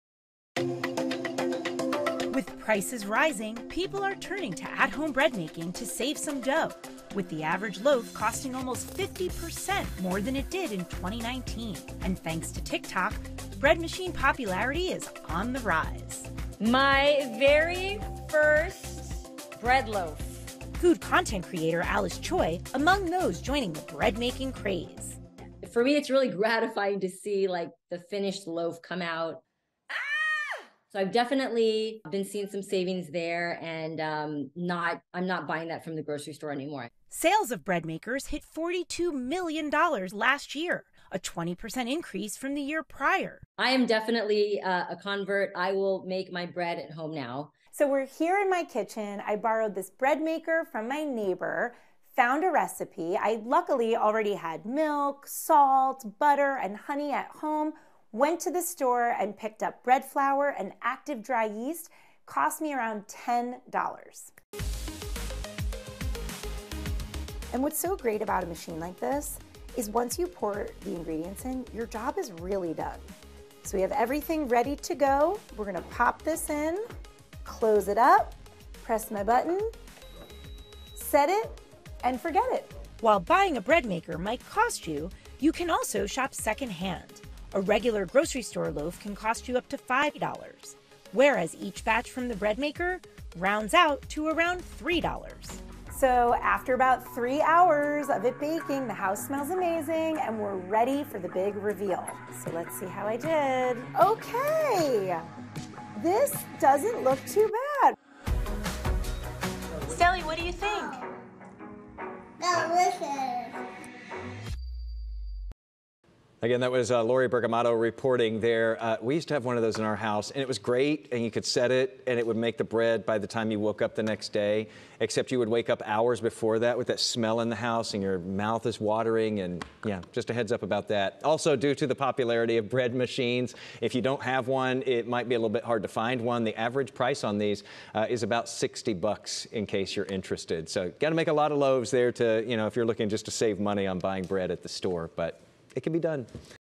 Bread machines becoming popular again
With food prices surging, a new device is becoming more popular in large part because of TikTok.